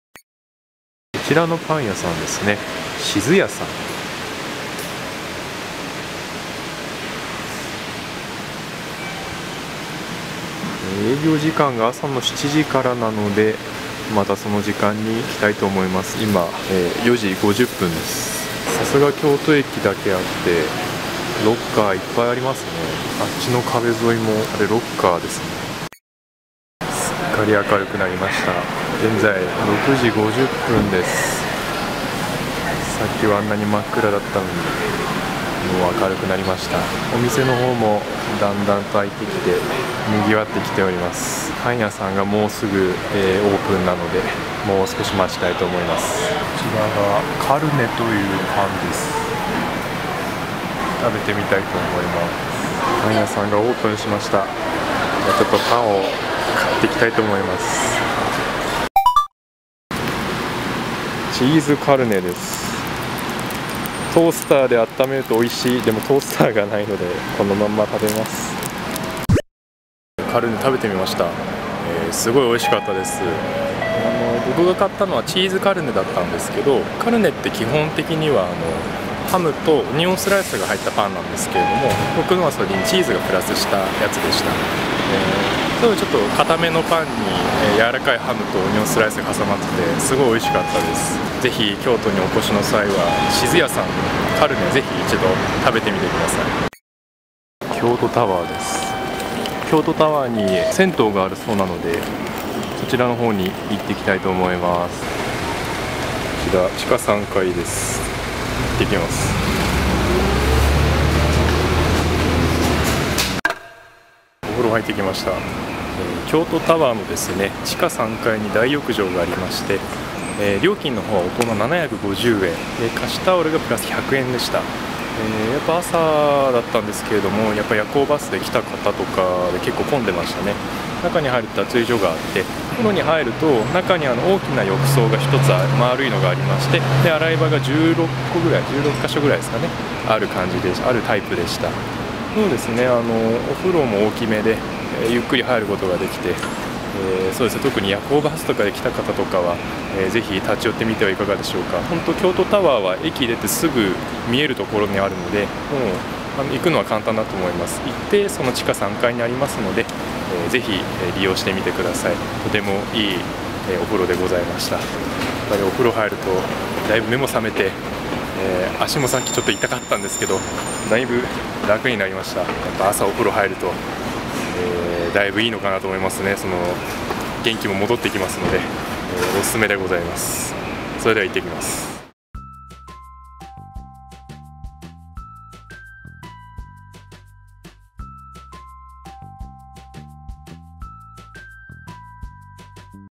【2014京都・大阪旅行】その2 名物パン＆銭湯編
何やら京都には知らない人は居ないというほど有名なパンがあるとの情報を得たため、実際に食べてみました！
また、深夜バスでの疲れを癒すために京都駅すぐ近くの銭湯へも行ってみました(*'▽')

Twitter アカウントを作成しました!

以上使用させていただきました。